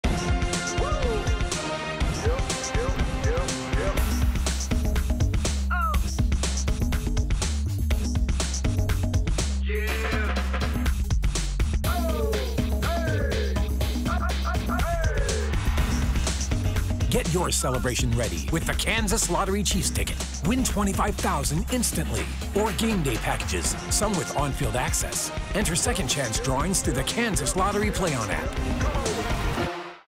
$5 Kansas City Chiefs Instant Scratch Ticket 2019 2.0
What does your victory dance look like? If you won $25,000 on the $5 Kansas City Chiefs instant scratch ticket, you'd definitely be dancing! Get your ticket now, and enter your non-winning tickets into the Kansas Lottery PlayOn app for a chance to win an Ultimate Chiefs Experience!